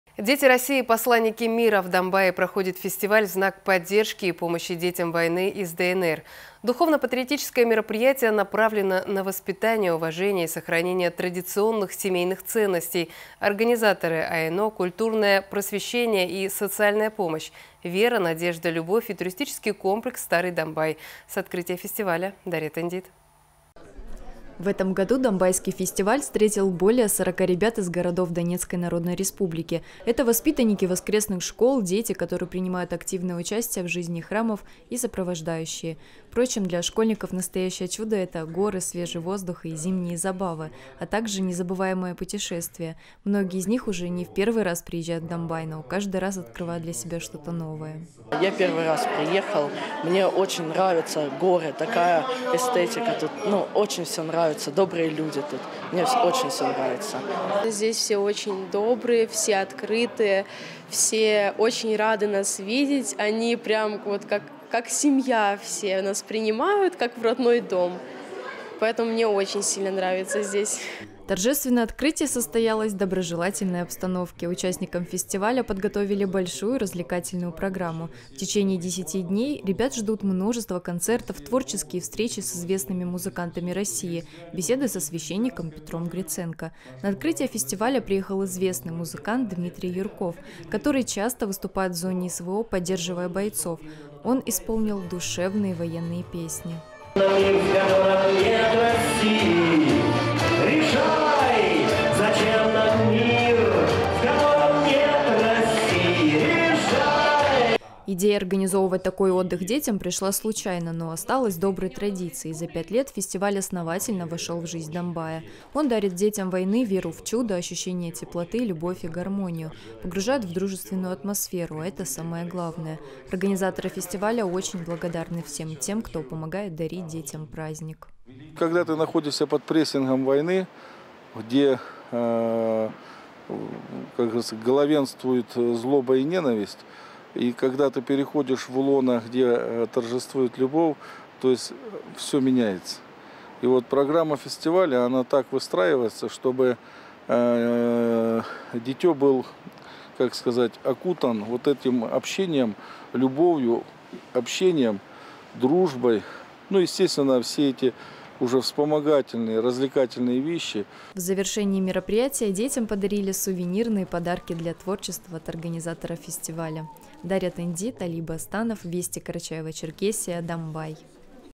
В Домбае проходит фестиваль в знак поддержки и помощи детям войны из ДНР
Дети России — посланники мира. В Домбае проходит фестиваль в знак поддержки и помощи детям войны из ДНР. Духовно-патриотическое мероприятие направлено на воспитание уважения и сохранение традиционных семейных ценностей